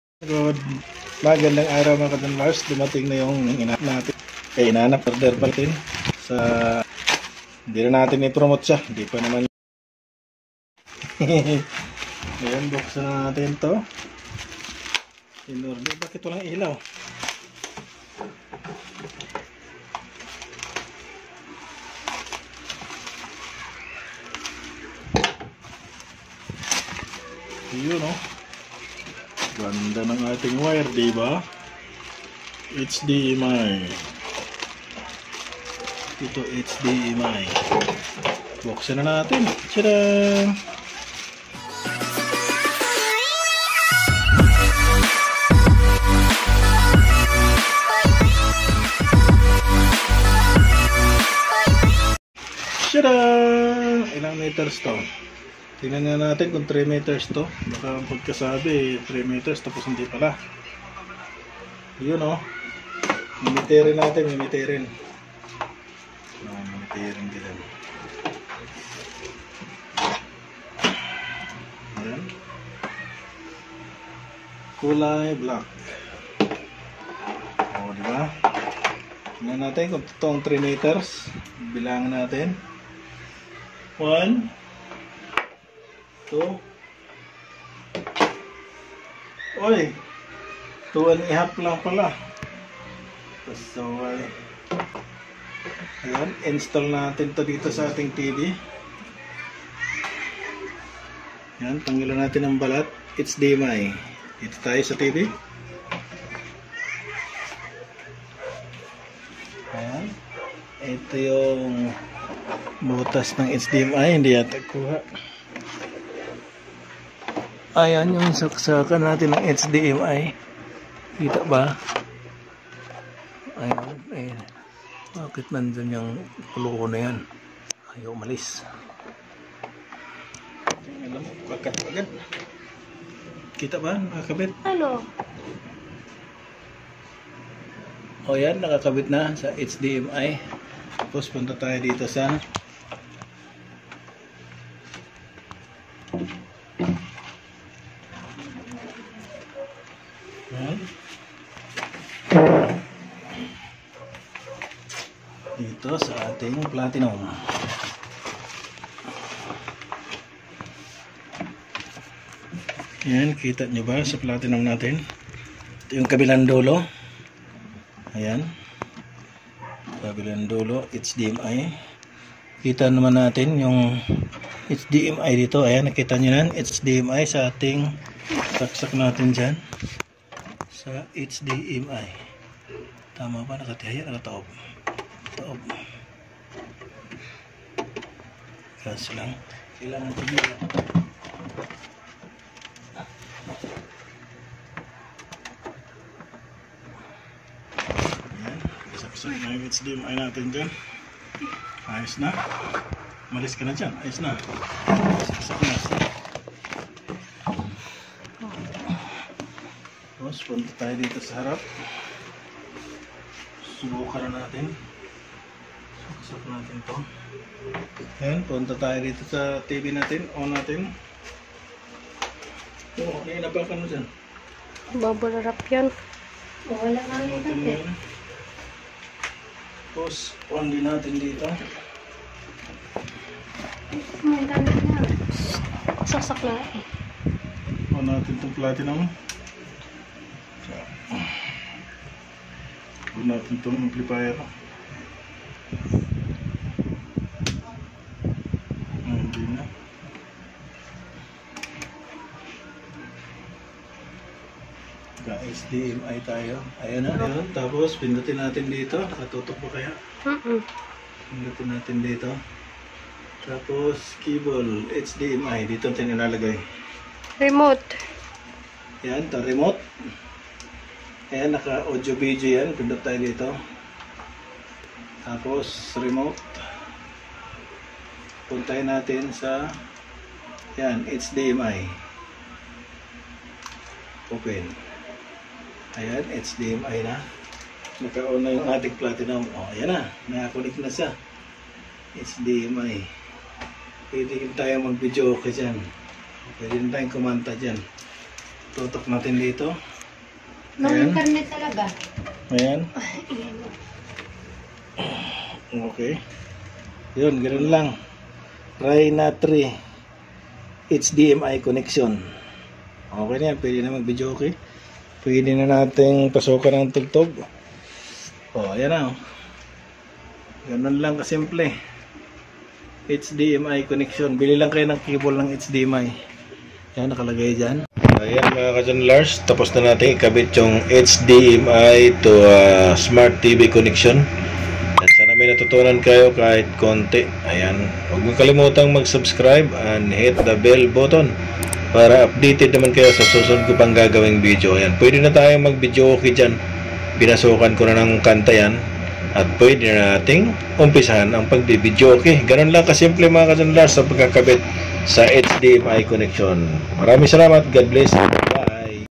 HDMI CONNECTION PLATINUM REYNA 3 TO SMART TV FOR VIDEOKE
Magandang araw mga ka junlars panibagong araw panibagong tutorial na naman tayo kong paano mag setup ng PLATINUM REYNA 3 HDMI CONNECTION TO SMART SKYWORTH TV.